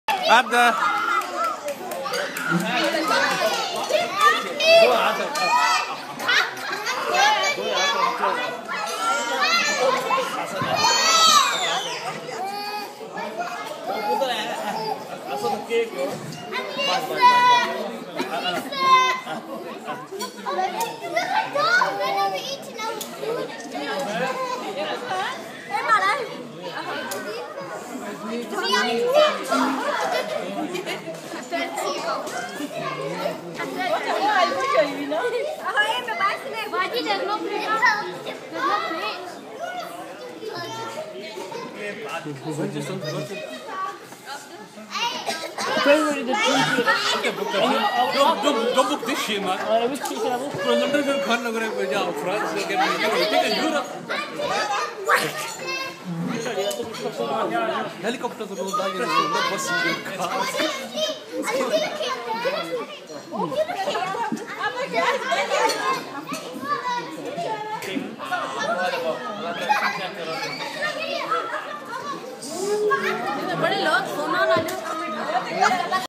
Zunairah Birthday 05052013
Zunairah birthday at Sunrise restaurant Bradford